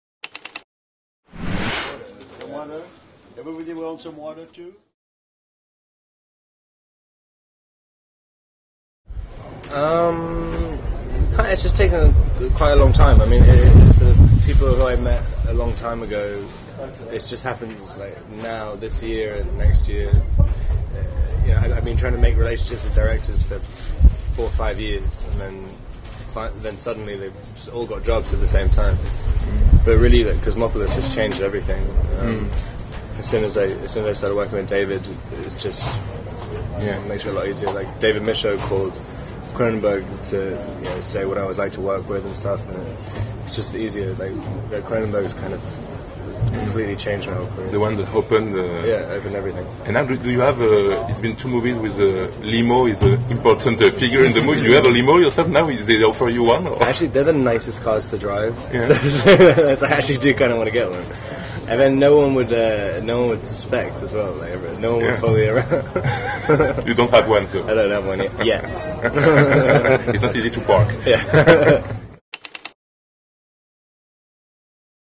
Robert Pattinson : "Non, je n'ai pas encore de limousine"
Rencontre avec Robert Pattinson au Festival de Cannes. (Images : Le Nouvel Observateur)